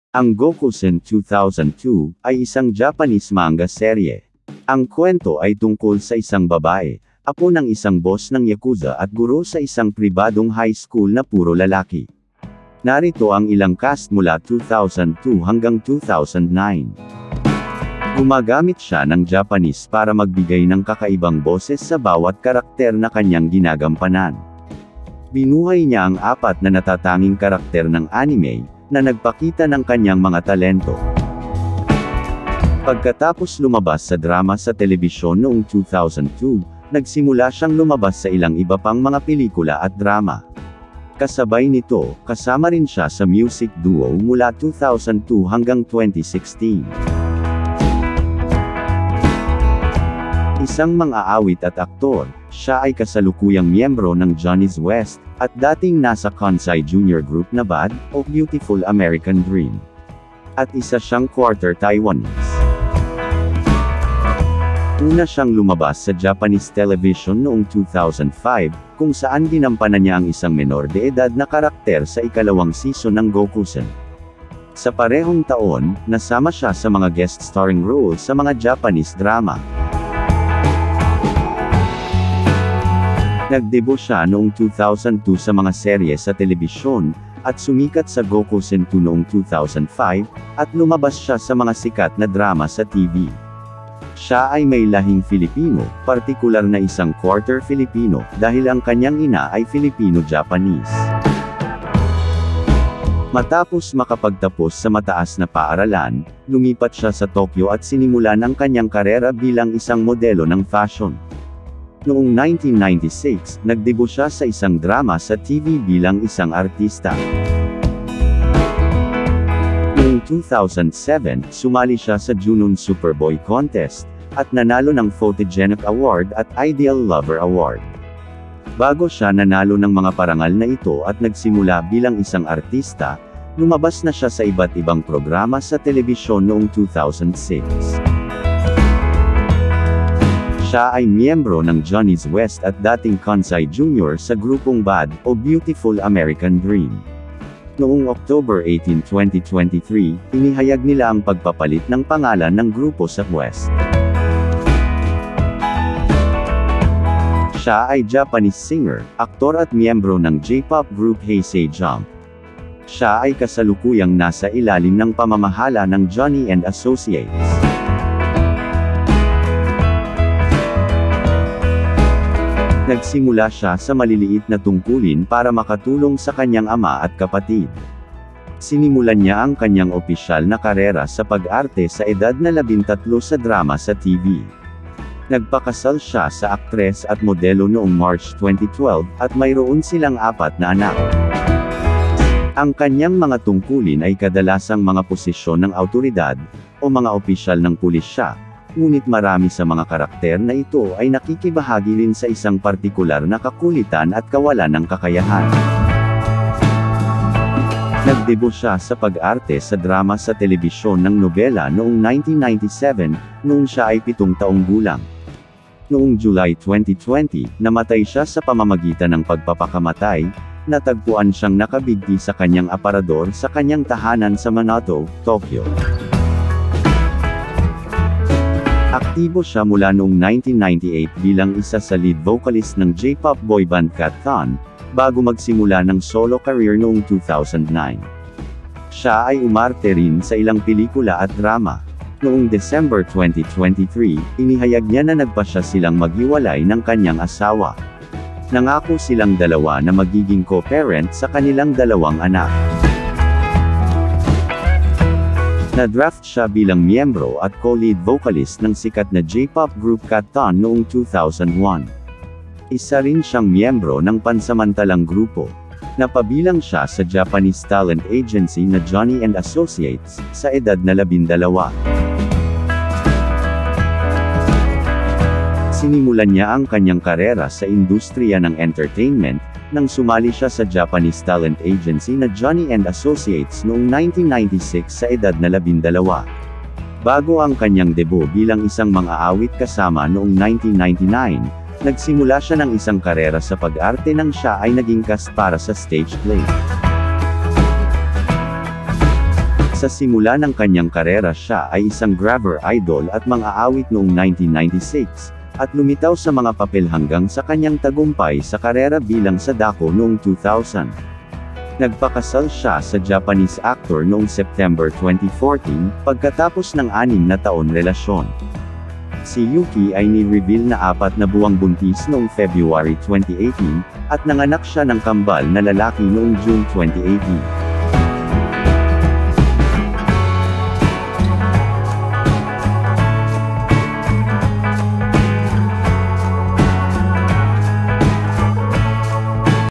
GOKUSEN 2002 Cast Then and Now 2024
Ang Gokusen 2002, ay isang Japanese manga serye. Ang kuwento ay tungkol sa isang babae, apo ng isang boss ng yakuza at guro sa isang pribadong high school na puro lalaki. Narito ang ilang cast mula 2002 hanggang 2009.